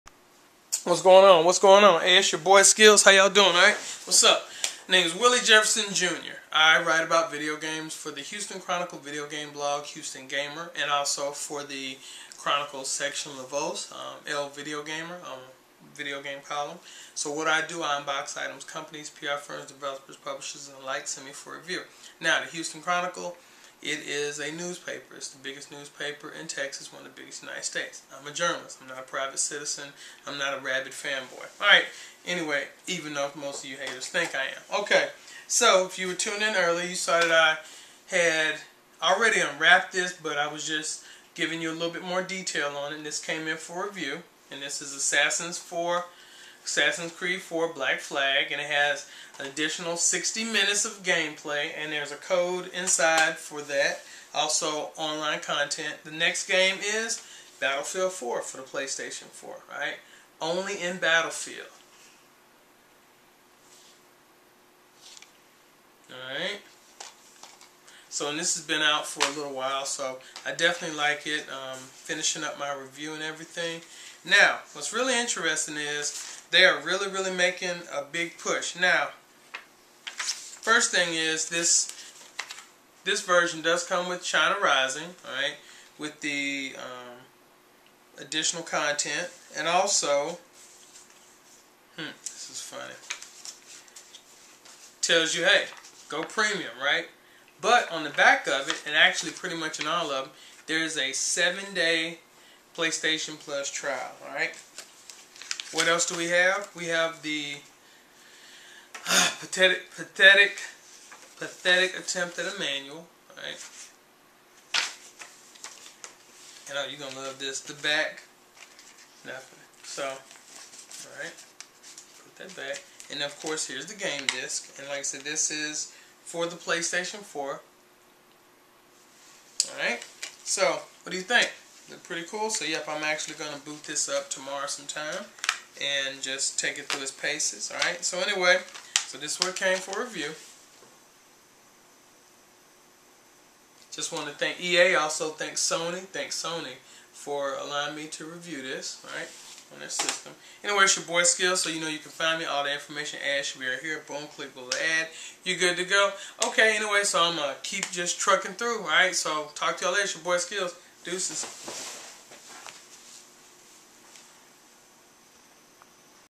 EARLY Battlefield 4 (PS4) Unboxing!
Here's the unboxing of Battlefield 4 for the PlayStation 4 and the PS4. I got Battlefield 4 at the PS4 Review Event/Launch Celebration in New York City on Monday.